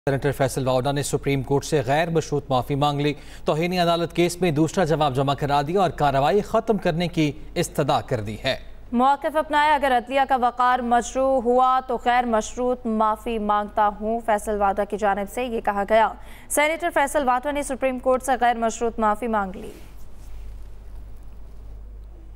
Faisal Vawda tenders ‘unconditional’ apology to Supreme Court | Pakistan News | Latest News
Senator Faisal Vawda has submitted an apology to the Supreme Court of Pakistan in a contempt of court case.

Vawda, in his second written response to the court, expressed his utmost respect for the judiciary.

Vawda stated, "Following the court proceedings on June 5, I met with religious scholars to understand the role of a senator. I was advised to stand by justice, even if it is against my own kin."

In his written apology, Vawda included references from the Qur'an and Hadith, demonstrating his sincerity.

He requested the Supreme Court to withdraw the show-cause notice issued to him, stating, "In the light of Islamic teachings, I seek forgiveness with a sincere heart.

If my press conference has harmed the dignity of the judiciary, I unconditionally apologise. I leave myself at the mercy of the court and request the withdrawal of the show-cause notice in the contempt case."

The contempt case against Vawda is set to be heard by a three-judge bench led by Chief Justice Qazi Faez Isa on June 28.